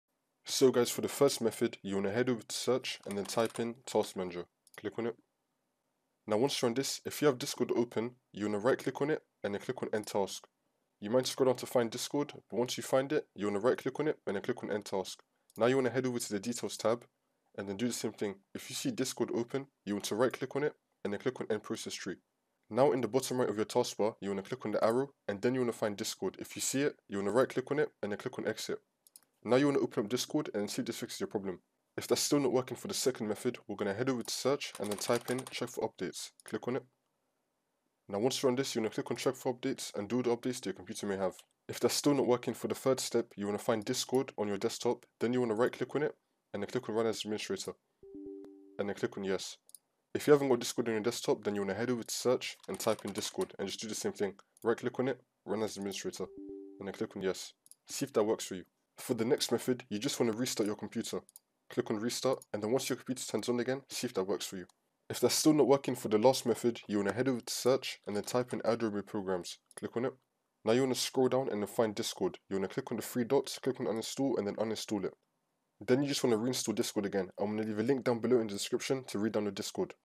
How To Fix Discord Not Opening on Windows 11 / 10
How To Fix Discord Not Opening on Windows 10/11

A short tutorial on fixing the problem when your Discord app is not properly opening on your Windows PC.